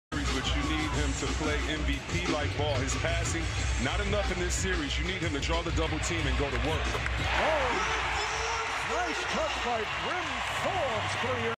NBA, playoff 2019, Nuggets vs. Spurs, Round 1, Game 4, Move 3, Bryn Forbes, 3 pointer
Denver Nuggets - San Antonio Spurs, Game 4, 20/04/2019, quarter 1
Bryn Forbes, 3 pointer